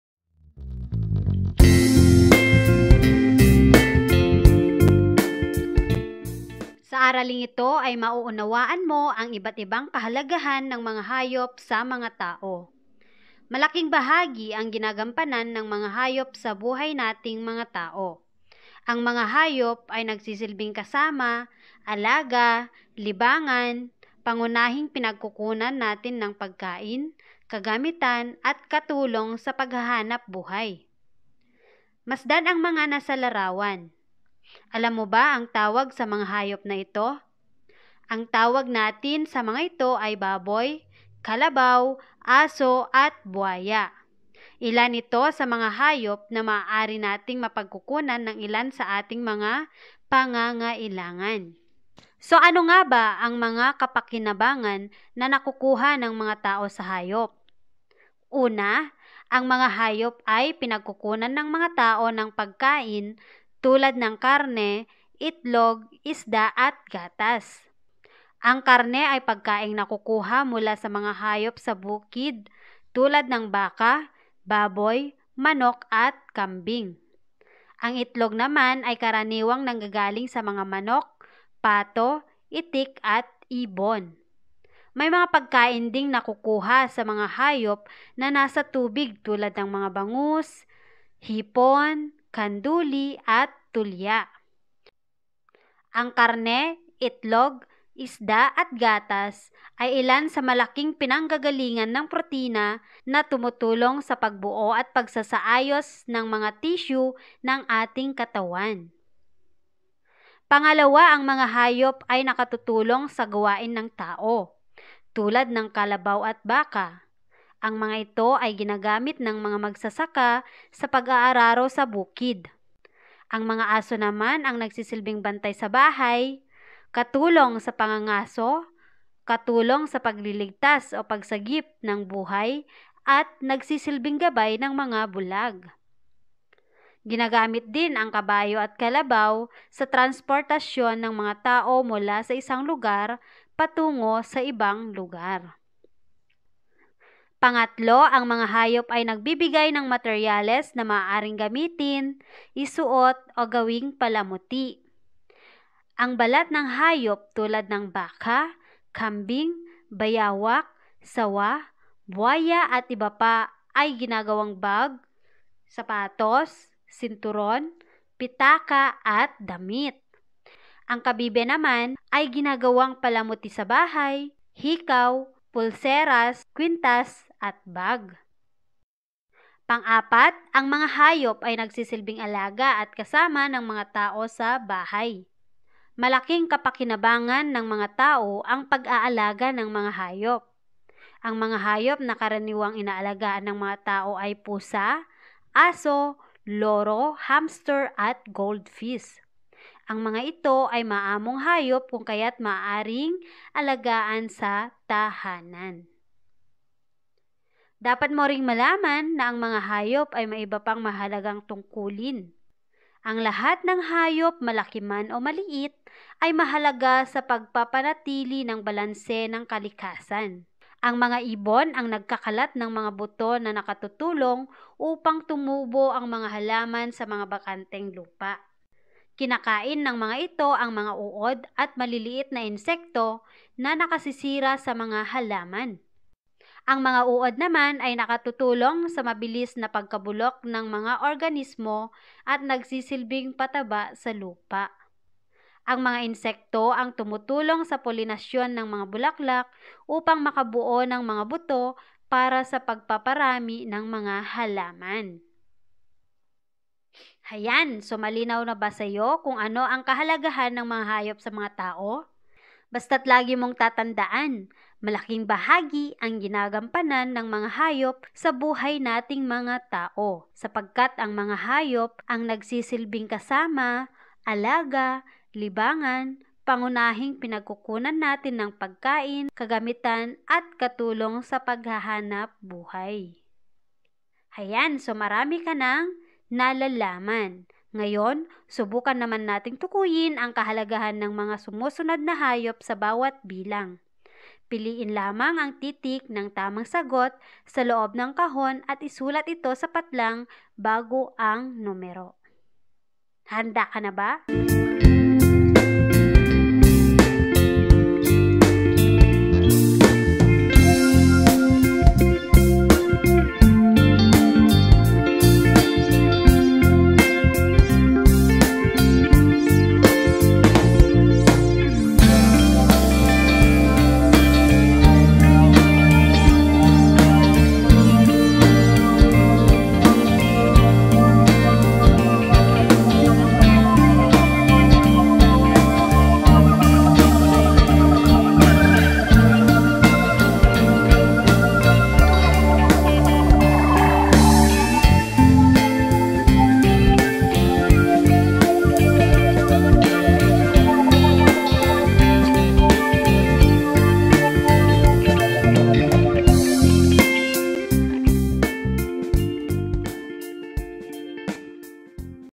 Kahalagahan ng Hayop sa Tao | with Activity |SCIENCE 3| QUARTER 2
Ang mga kapakinabangan na nakukuha ng mga tao sa hayop.

1. Ang mga hayop ay pinagkukunan ng mga tao ng pagkain tulad ng karne, itlog, Isda at gatas. Ang karne ay pagkaing nakukuha mula sa mga hayop sa bukid tulad ng báka, baboy, manok, at kambing. Ang itlog naman ay karaniwang nanggagaling sa manok, pato, itik at ibon.   May mga pagkain ding nakukuha sa mga hayop na nása tubig tulad ng mga bangus, hipon, kanduli, at tulya. Ang mga ito ang malaking pinanggagalingan ng protina na tumutulong sa pagbuo at pagsasaayos ng mga tissue ng ating katawan.

2. Ang mga hayop ay nakatutulong sa gawain ng tao. Tulad ng kalabaw at báka, ang mga ito ay ginagamit ng mga magsasaka sa pag-aararo sa bukid. Ang mga aso naman ang nagsisilbing bantay sa bahay, katulong sa pangagaso, katulong sa pagliligtas o pagsagip ng búhay, at nagsisilbing gabay ng mga bulag. Ginagamit din ang kabayo at kalabaw sa transportasyon ng mga tao mula sa isang lugar patungo sa ibang lugar.

3.  Ang mga hayop ay nagbibigay ng materyales na maaaring gamitin, isuot o gawing palamuti. Ang balat ng hayop tulad ng baka, kambing, bayawak, sawa, buwaya at iba pa ay ginagawang bag, sapatos, sinturon, pitaka at damit. Ang kabibe naman ay ginagawang palamuti sa bahay, hikaw, pulseras, kwintas at bag.

4. Ang mga hayop ay nagsisilbing alaga at kasama ng mga tao sa bahay. Malaking kapakinabangan ng maraming tao ang pagaalaga ng mga hayop. Ang mga hayop na karaniwang inaalagaan ng mga tao ay pusa, aso, loro, hamster at goldfish. Ang mga ito ay maaamong hayop kung kayat maaaring alagaan sa tahanan.

5. Ang mga hayop ay may iba pang mahalagang tungkulin.
Ang lahat ng hayop, malaki man o maliit, ay mahalaga sa pagpapanatili ng balanse ng kalikasan. Ang mga ibon ang nagkakalat ng mga buto na nakatutulong upang tumubo ang mga halaman sa mga bakanteng lupa. Kinakain ng mga ito ang mga uod at maliliit na insekto na nakasisira sa mga halaman. Ang mga uod naman ay nakatutulong sa mabilis na pagkabulok ng mga organismo at nagsisilbing pataba sa lupa. Ang mga insekto ang tumutulong sa polinasyon ng mga bulaklak upang makabuo ng mga buto para sa pagpaparami ng mga halaman.